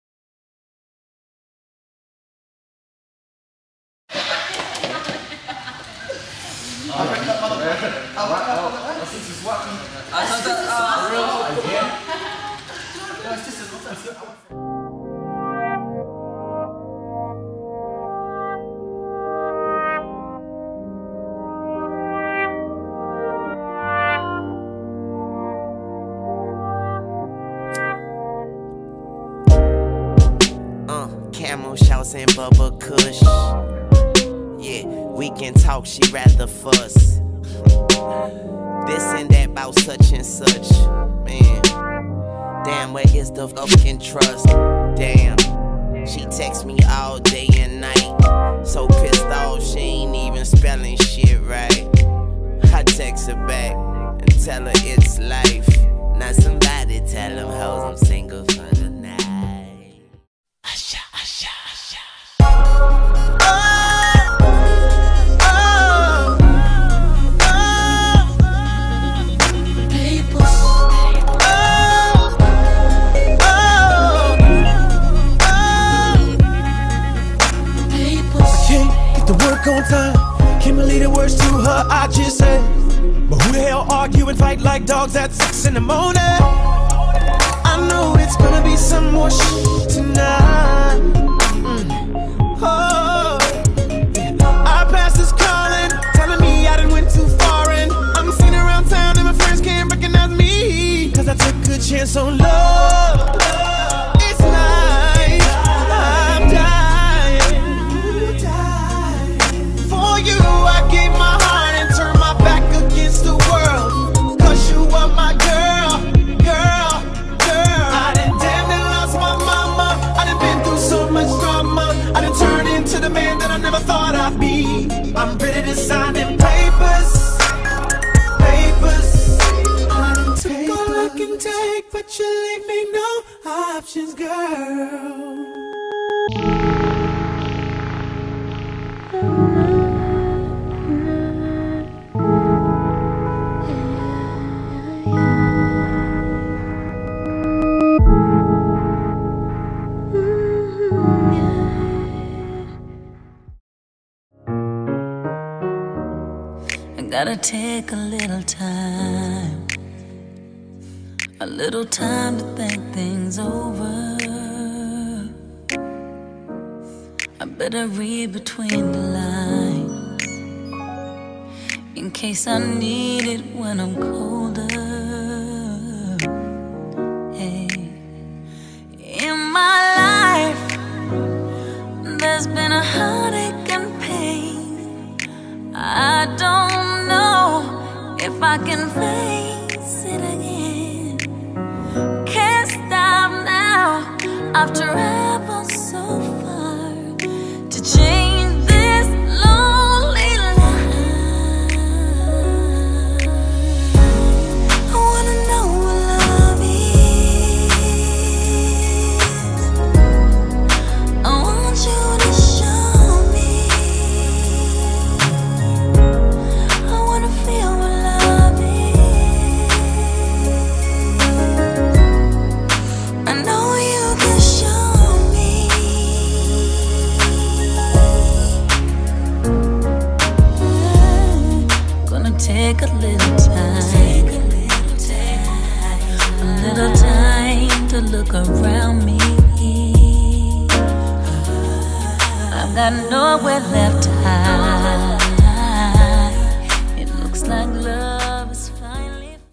Phillip Geniza - Papers (Short Film HDK)
Chris Baterina & Raniel Macasaet
a HDK production

Concept and Choreography
Phillip Geniza

Dancers
Jun Quemado
Pat Lam
Jackie Lautchang
Andrew Baterina

Soreal Dance Studio

**Special Thanks goes out to Jun Q, SOreal Cru, and HDK for making this vid possible. Follow all of their work, because they are such talented and down to earth people. LIVE and FEEL through dance.

Happy Holidays!

#53 - Top Favorited (This Month) - Entertainment